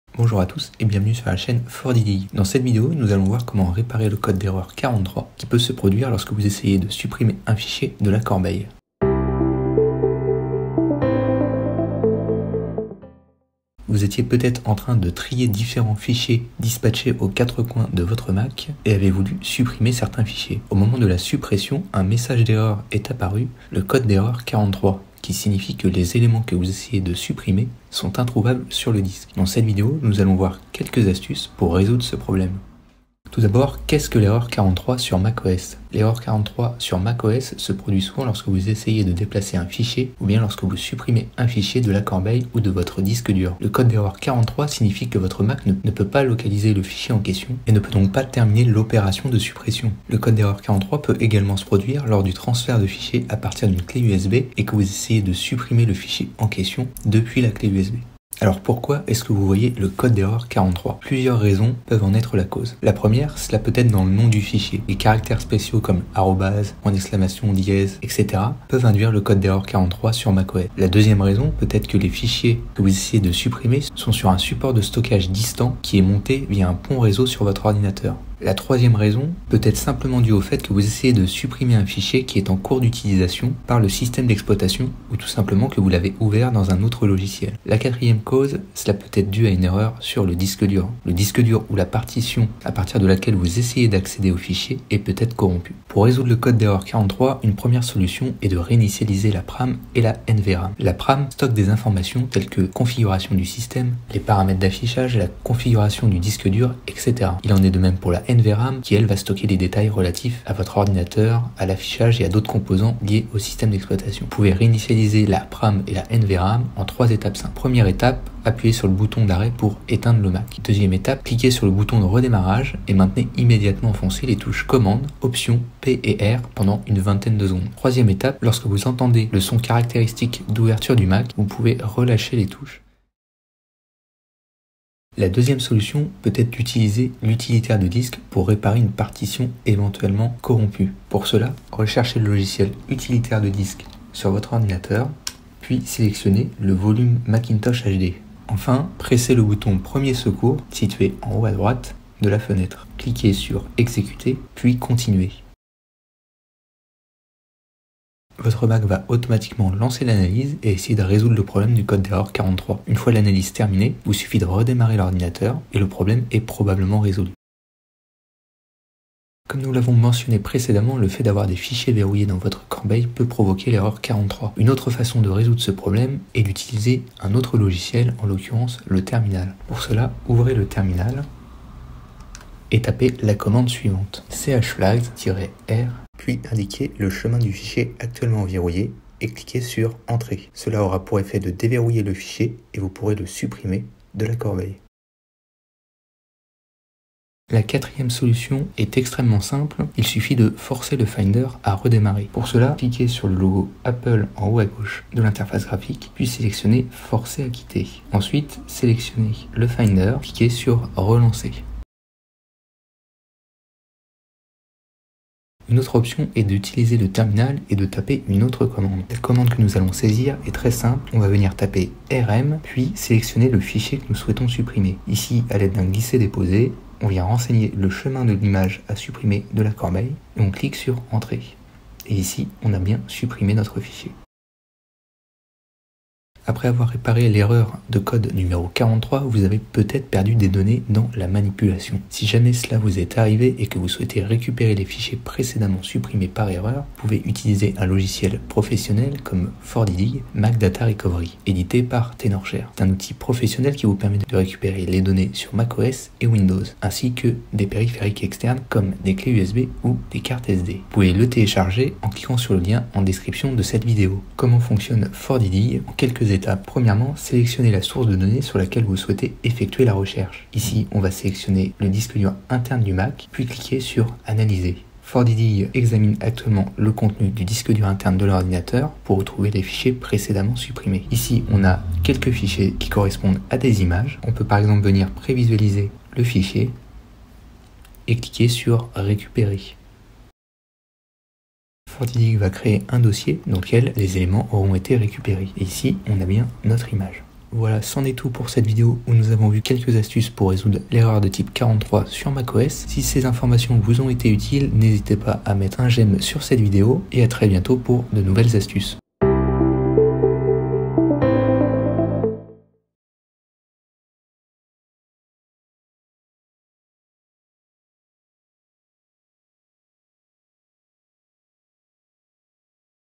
[5 Méthodes] Réparer le code d'erreur 43 sur Mac 🛠
Téléchargez Tenorshare 4DDiG pour récupérer les données perdues ou supprimées sur Mac et Windows

Le code d'erreur 43 se produit souvent lorsque vous essayez de déplacer un fichier, ou lorsque vous supprimez un fichier de la corbeille ou du stockage. Le code d'erreur signifie que Mac ne peut pas localiser le fichier en question et ne peut donc pas terminer l'opération. Le code d'erreur 43 peut également se produire lors du transfert de fichiers à partir d'une clé USB ou de la suppression de fichiers à l'intérieur d'une clé USB. Dans cette vidéo, on va vous présenter 5 méthodes efficaces.

📌Timestamp
00:00 Introduire
00:34 Qu'est-ce que le code d'erreur 43 sur Mac
00:59 Pourquoi est-ce je vois le code d'erreur 43
01:37 Méthode 1 : Réinitialiser la PRAM / NVRAM
02:14 Méthode 2 : Vérifier l'Utilitaire de disque
02:48 Méthode 3 : Supprimer des fichiers verrouillés
03:17 Méthode 4 : Quitter le Finder de force
03:35 Méthode 5 : Utiliser les commandes du Terminal
03:55 Comment récupérer des données perdues après le code d'erreur 43👍

Si vous voulez savoir plus de solutions ou des outils pour récupérer les données sur votre ordinateur, ne manquez pas les articles suivantes :

✅Que faire si vous ne pouvez pas décompresser le fichier 7-Zip sur Mac